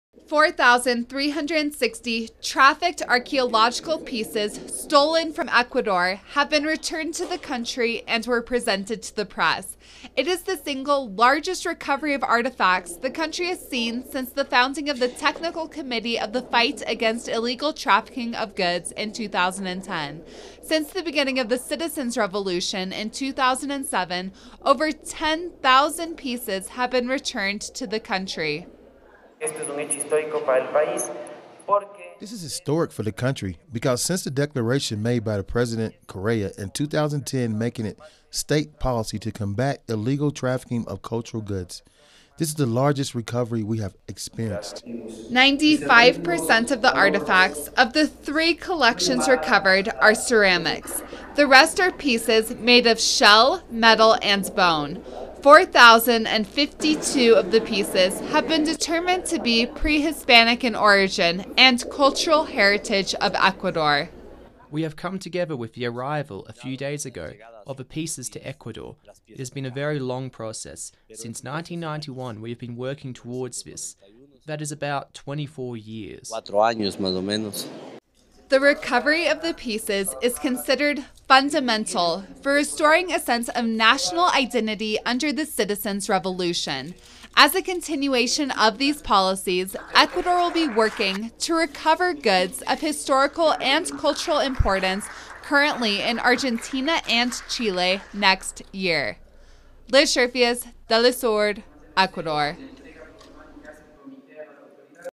Reportaje de Telesur sobre las más de cuatro mil piezas arqueológicas recuperadas
El pasado año se realizó la repatriación de más de cuatro mil piezas arqueológicas que habían salido del país a través del tráfico ilícito.
Entérate más en este reportaje que es cortesía de Telesur.